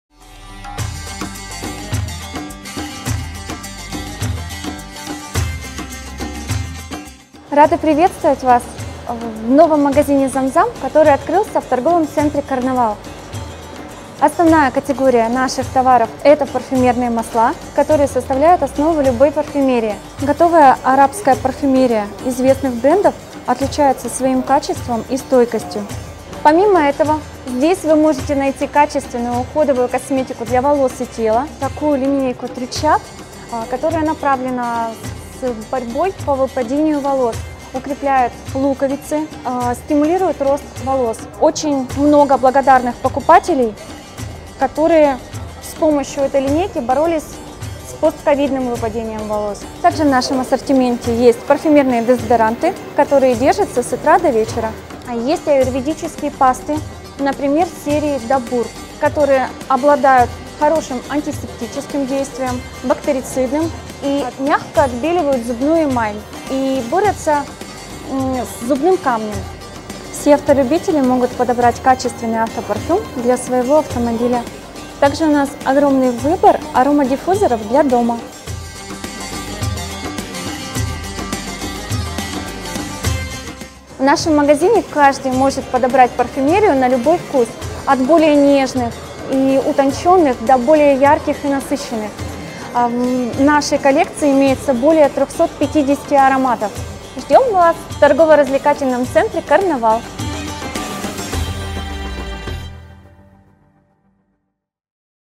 Zam zam в ТРЦ Карнавал
Отличные новости! На цокольном этаже ТРЦ "Карнавал" открылся магазин-остров Zam-Zam - территория элитных ароматов!

Zam-zam - это элитные парфюмерные масла. Главное преимущество духов, созданных на основе концентрированного масла - стойкость. Ваш любимый аромат будет с вами до 3х дней! Вы можете использовать масло в чистом виде или в виде спрея (концентрация масла в готовом спрее 50%!).

В магазине вы сможете купить аюрведическую косметику. Например, индийский шампунь Тричап поможет решить проблему с выпадением волос, ускорит рост волос, устранит хрупкость и ломкость.

В продаже есть парфюмированные дезодоранты, ароматы для дома и автомобиля и многое другое. Убедитесь сами! Приглашаем в новый магазин Zam-Zam, цокольный этаж ТРЦ "Карнавал"!